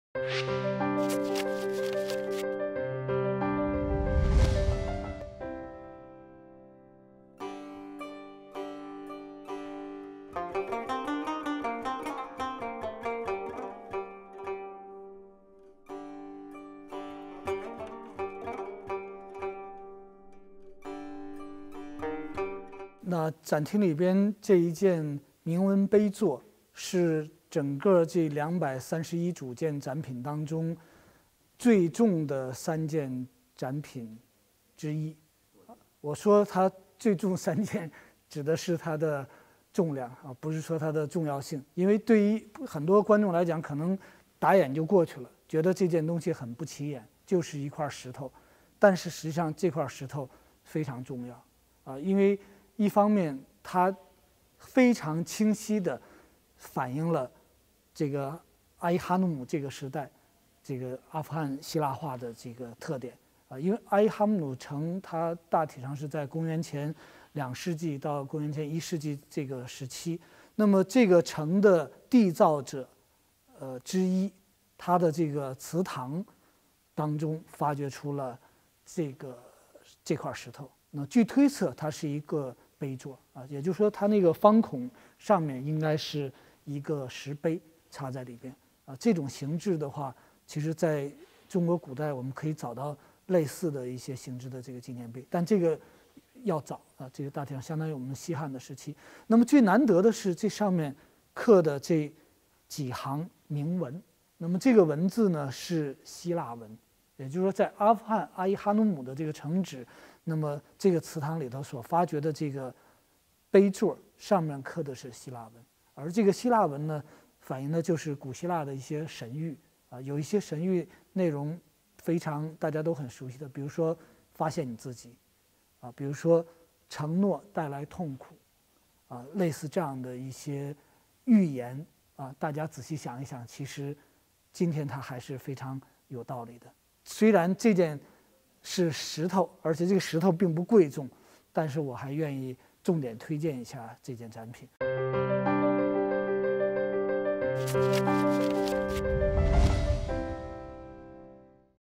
可能是最早的“心灵鸡汤”！2000年前阿富汗的石头让你认识你自己
人文清华短视频： 清华大学艺术博物馆展出《器服物佩好无疆——东西文明交汇的阿富汗国家宝藏》展览，展览上的一个铭文碑座成为了最“重”的一件展品，上面刻有古代“心灵鸡汤”。